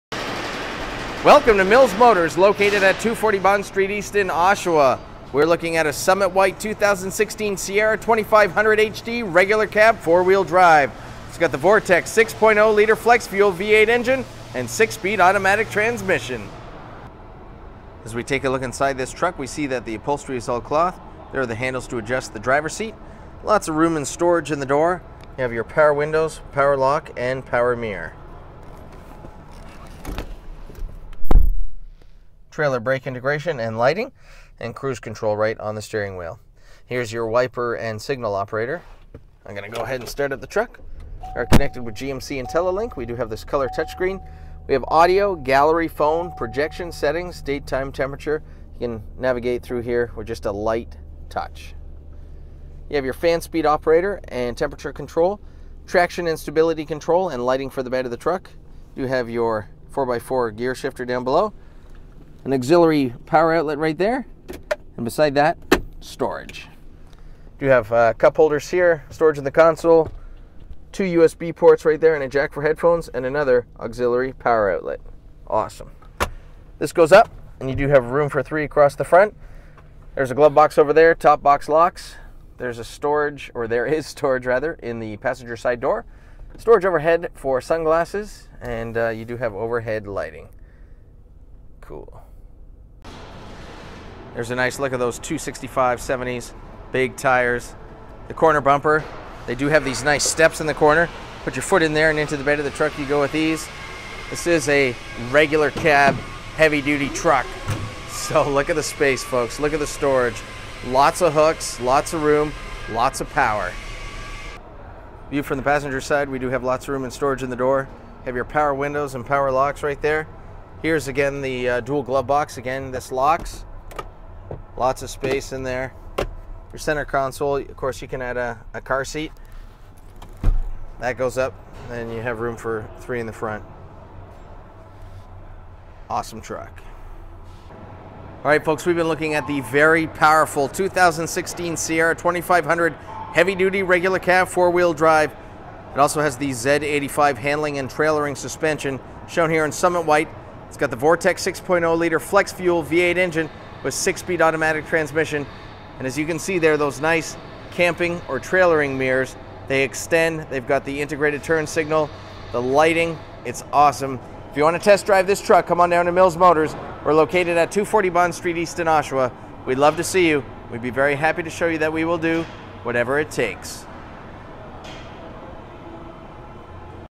2016 GMC Sierra 2500HD Regular Cab 4WD 6.0L FlexFuel V8 Oshawa ON Stock # 160462
This is a 2016 GMC Sierra 2500HD with 6-Speed A/T transmission White[Summit White] color and Jet Black interior color.  This video is recorded and uploaded by cDemo Mobile Inspector